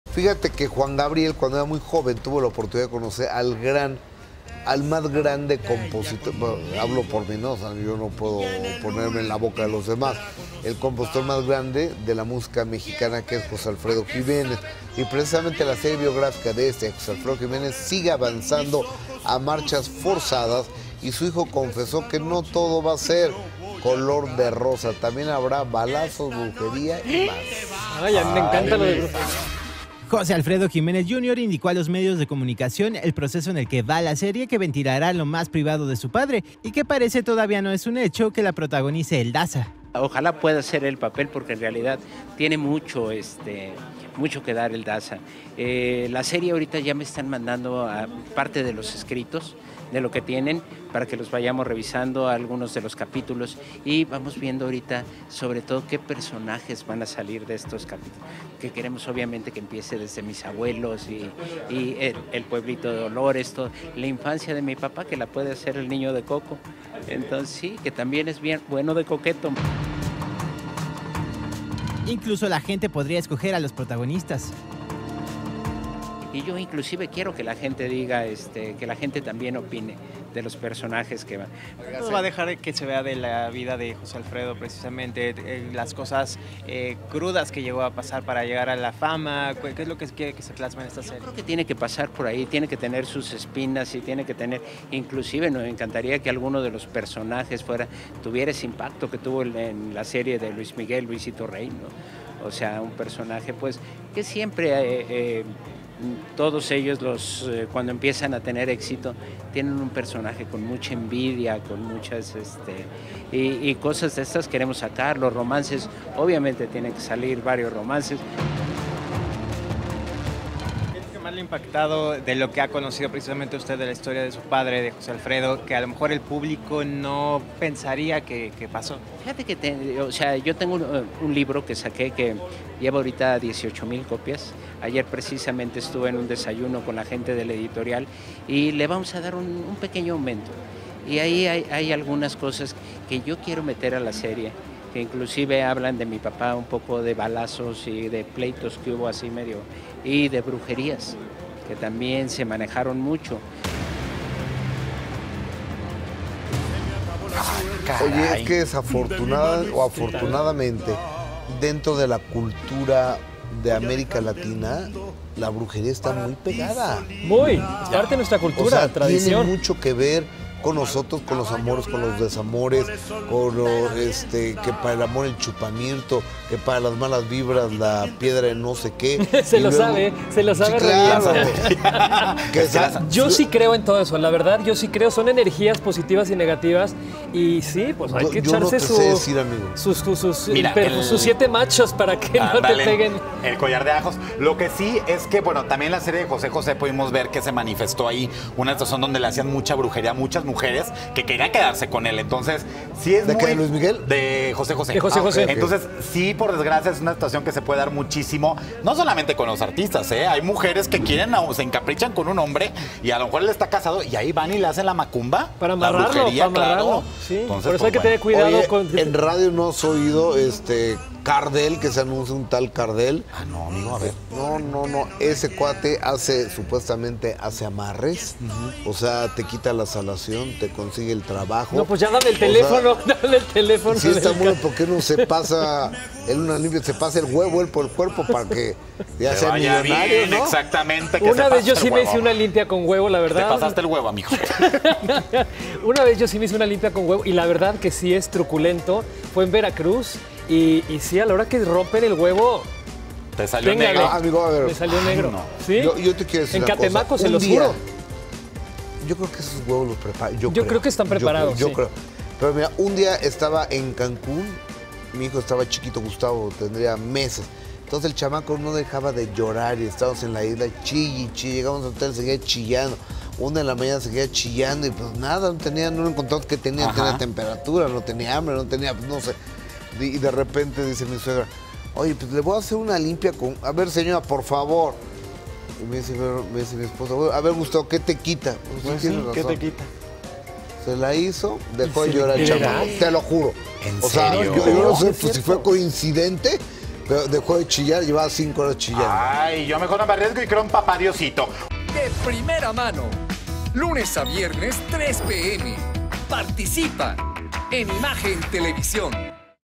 Niño de "Coco" podría interpretar a José Alfredo Jiménez en bioserie | De Primera Mano
El hijo de José Alfredo Jiménez revela los secretos que podrían aparecer en la bioserie de su padre.